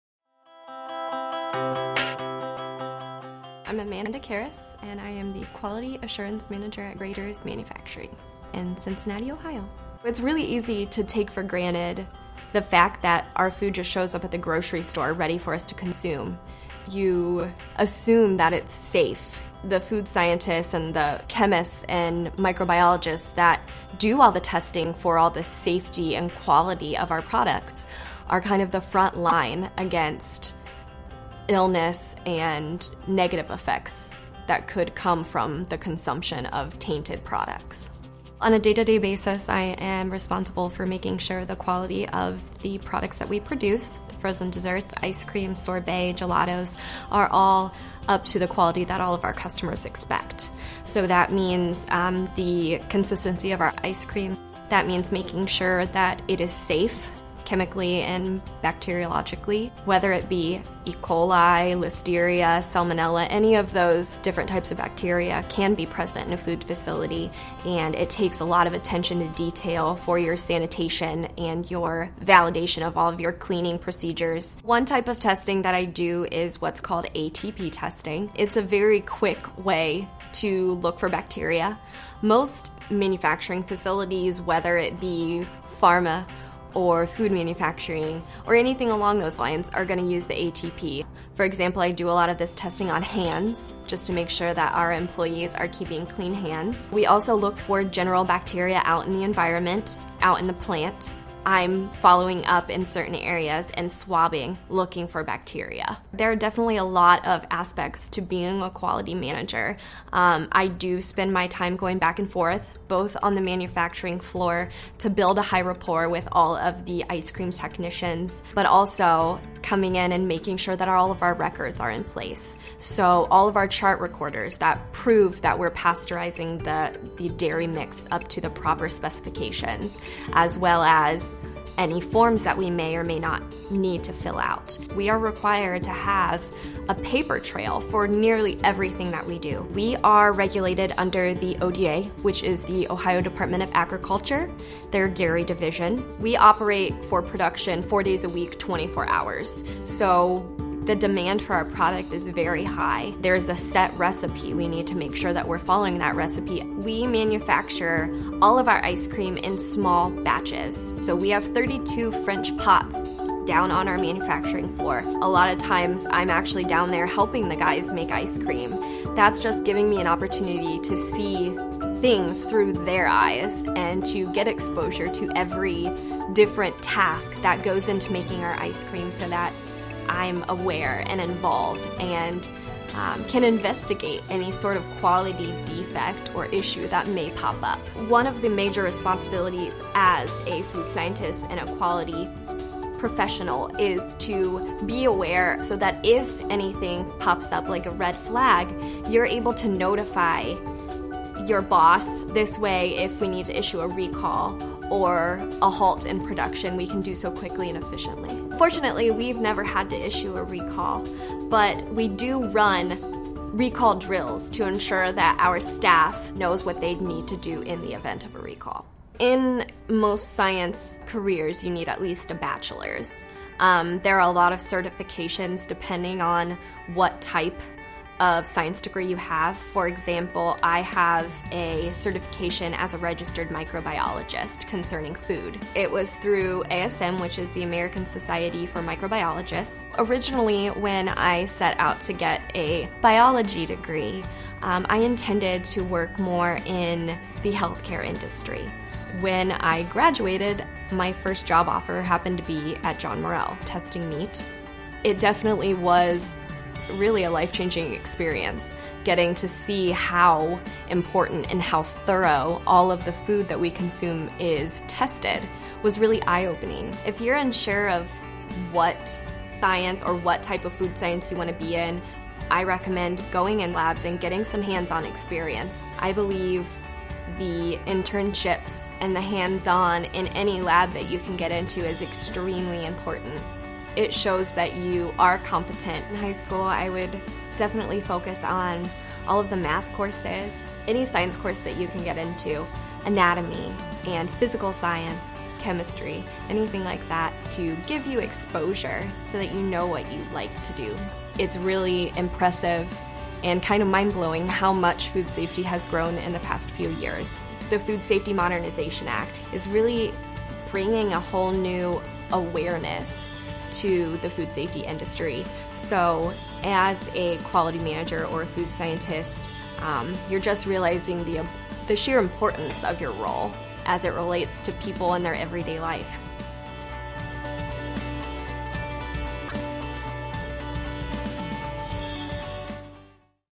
Graeter's Ice Cream How it’s Made
Ice cream and science.  What could go together better? Learn how high school is the time to begin preparing for a rewarding career that keeps our food factories clean and consumers safe and healthy.